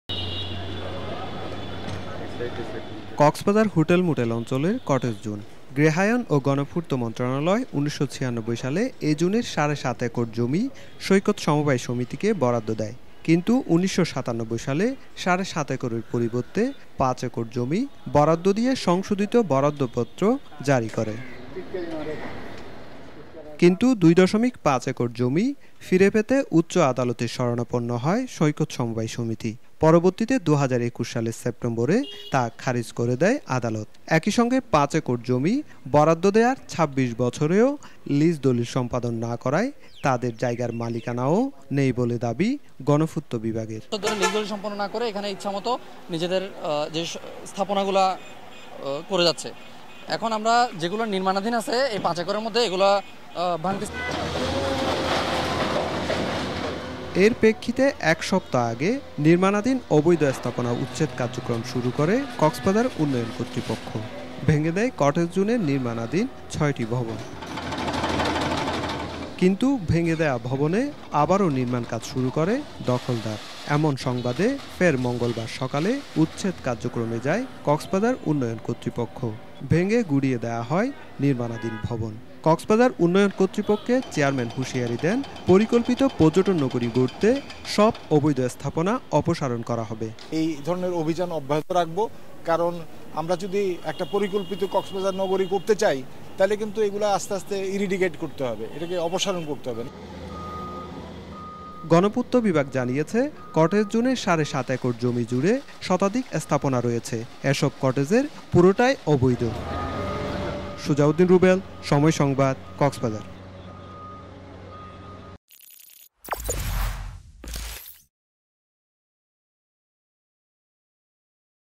কক্সবাজার কটেজ জোনে অবৈধ স্থাপনা উচ্ছেদ | Cox's Bazar | Somoy TV
পরিকল্পিত পর্যটন নগরী গড়ে তুলতে কক্সবাজারে কটেজ জোনে বেশ কয়েকটি অবৈধ স্থাপনা গুঁড়িয়ে দিয়েছে প্রশাসন।